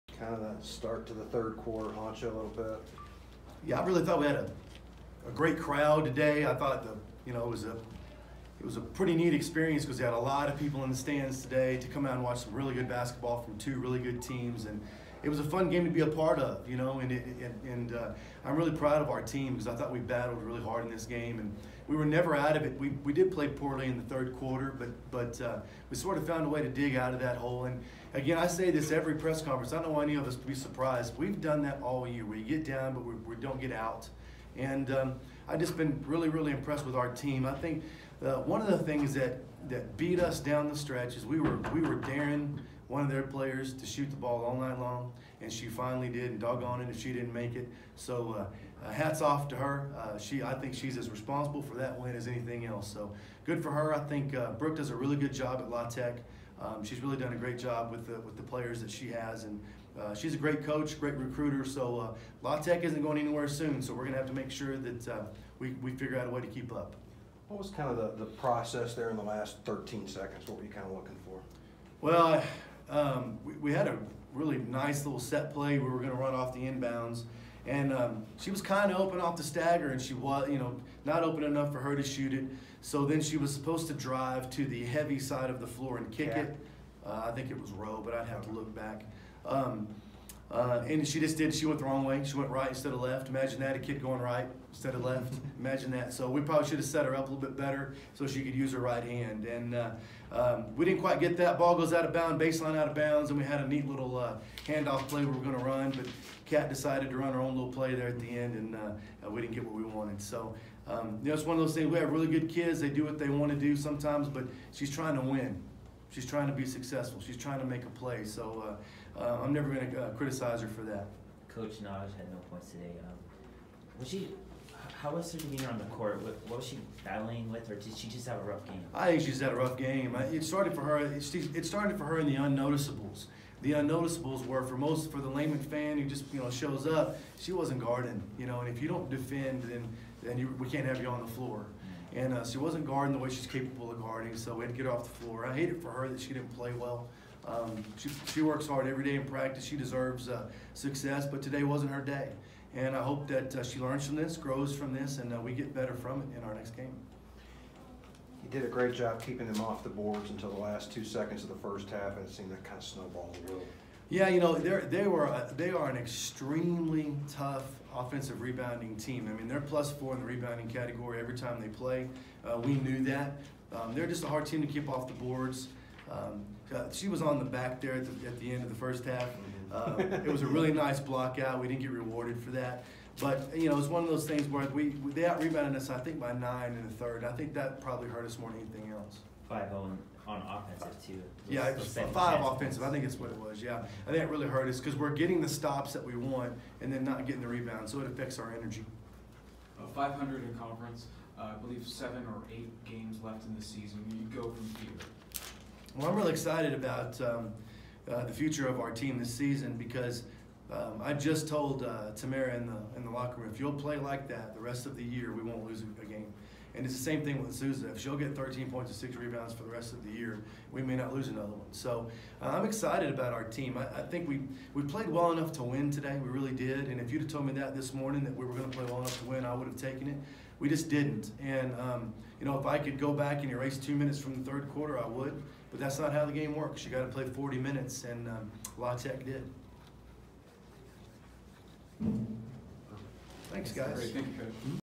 UTEP Women’s Basketball vs LA Tech: Kevin Baker Postgame Press Conference (2-10-18)
UTEP head coach Kevin Baker addresses the media following a 65-62 loss to Louisiana Tech.

Miners fall to 14-9 overall and 5-5 in C-USA play.